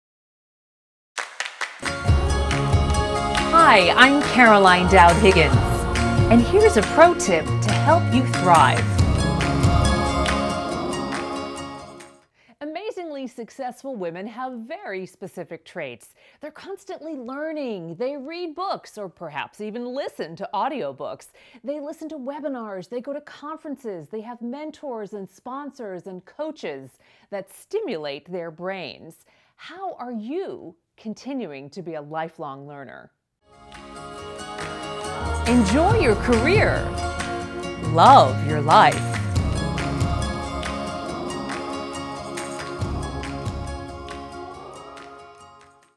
Be a Lifelong Learner to Stimulate Your Intellectual Curiosity
Take advantage of books, webinars, coaches and mentors to continue learning throughout your life and career. More Caroline Dowd-Higgins Podcasts and Videos on iTunes